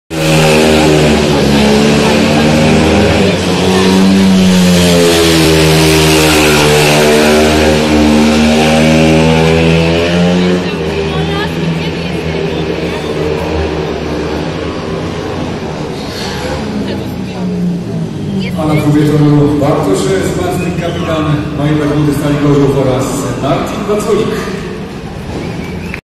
Sprawdzanie Toru Przez Zawodników Stali Gorzów Kapitan Drużyny Bartosz Zmarzlik I Martin Vaculik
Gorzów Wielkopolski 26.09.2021